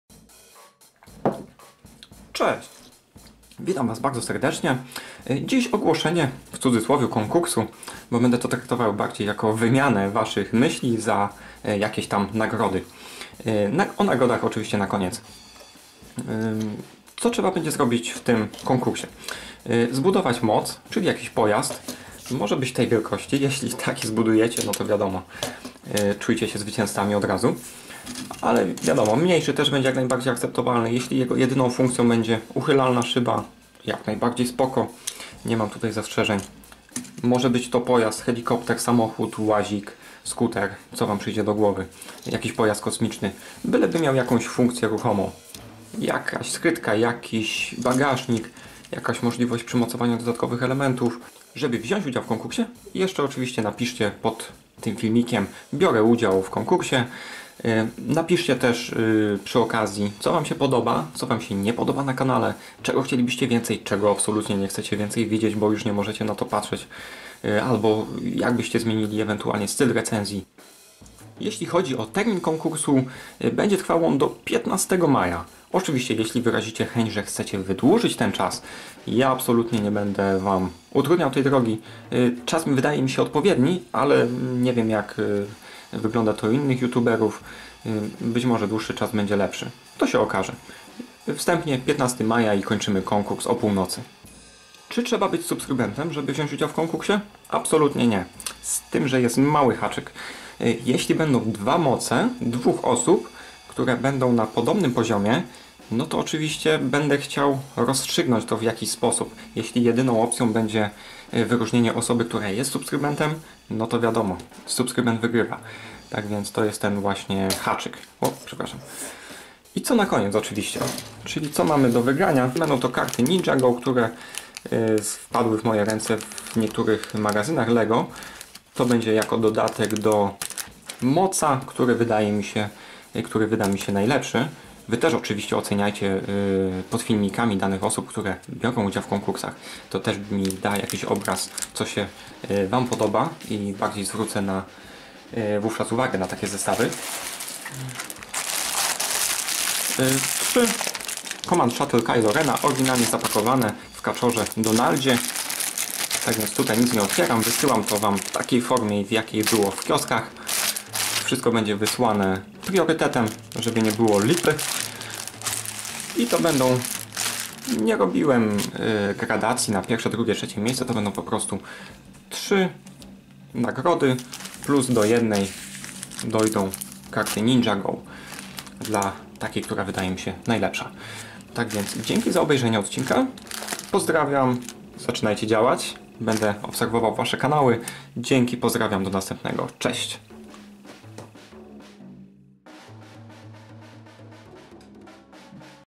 Konkurs (termin do 30 maja , dopuszczalne budowle z klocków Lego i Cobi!)
Zasady „konkursu” 
Termin: Do 30 maja do północy (lub dłużej na życzenie Widzów). Do tego czasu czekam na komentarze pod filmem potwierdzające Twój udział w konkursie o treści „Biorę udział w konkursie”
W tym samym komentarzu napisz jeszcze drogi Widzu co chcesz zmienić, czego chcesz widzieć więcej, a co mam wyrzucić z ramówki 
Kolejny wymóg konkursu – budowa MOC – pojazdu. Liczba klocków do 150. Może to być samochód, łazik, skuter, samolot, łódź, pojazd kosmiczny itd.
Moje wymagania to opływowy kształt, symetria, nie zaburzone proporcje oraz elementy ruchome/chowane w kadłubie, elementy transformacji itp.
Wielkość pojazdu nie ma głównego znaczenia, małe będę oceniał podobnie do dużych.
Do wygrania polybag Kylo Ren’s Command Shuttle x3, do jednego z nich dodam karty Ninjago.
Ogłoszenie wyników nastąpi najpóźniej do połowy czerwca (15.06).
Czas ogłoszenia wyników może się wydłużyć o ile nastąpi przedłużenie terminu „nadsyłania” czy tworzenia prac na konkurs.
Kwestie sporne regulaminu rozstrzyga Chuck Norris i Johnny Thunder,
Pozdrawiam 